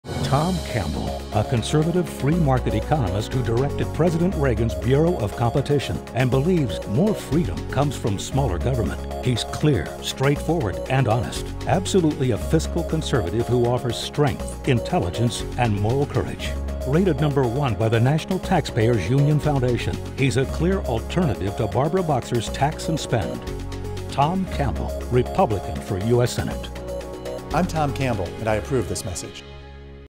Clear Alternative
Tom Campbell is a conservative free market economist, and believes that more freedom comes from smaller government.